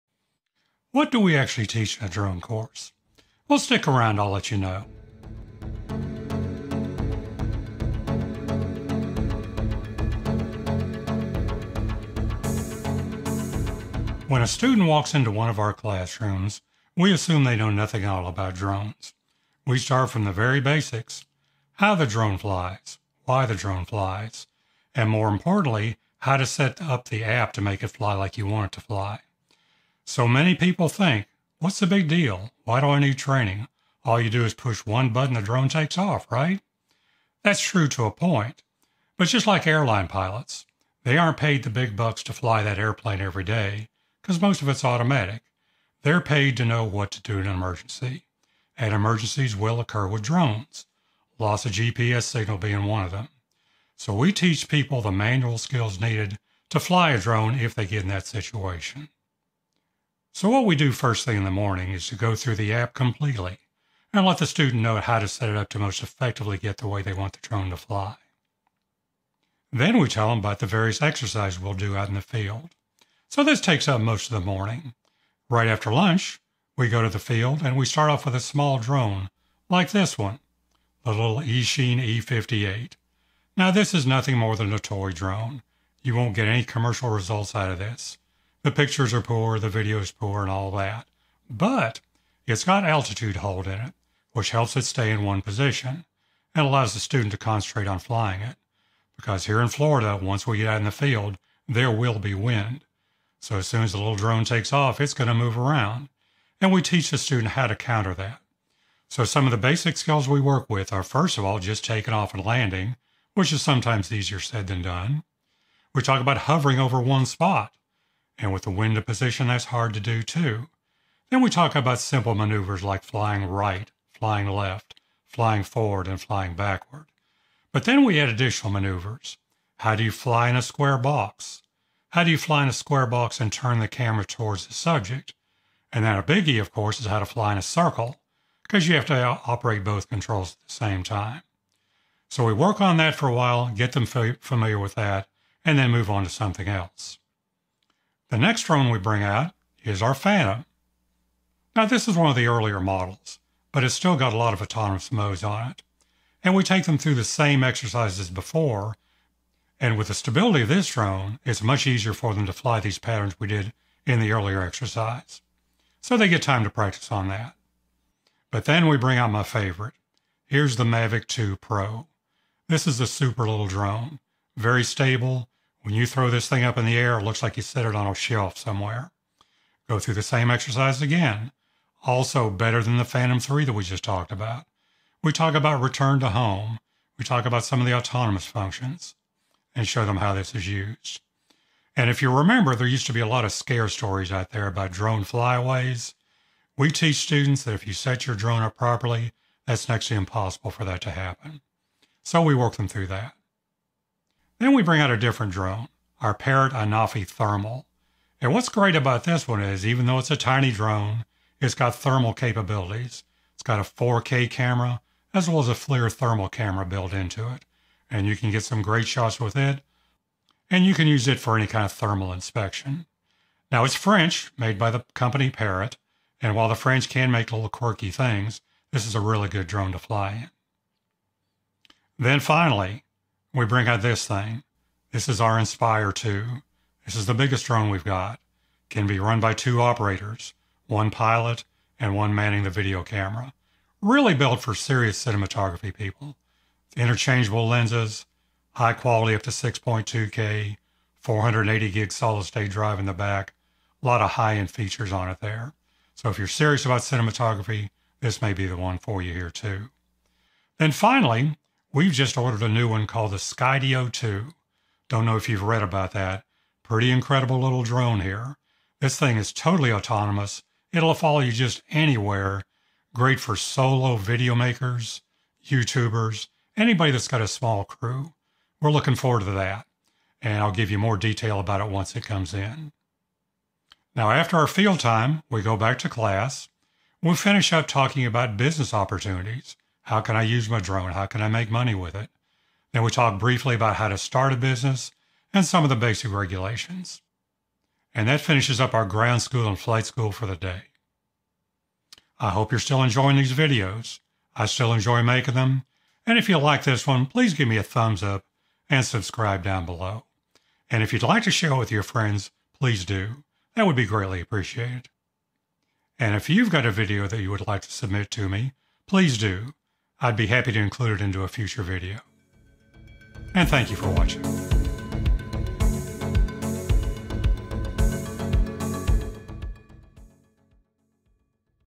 What do you Learn in One of our Drone Classes
This week, I talk about a typical day in our Drone Ground/Flight school and what a student will learn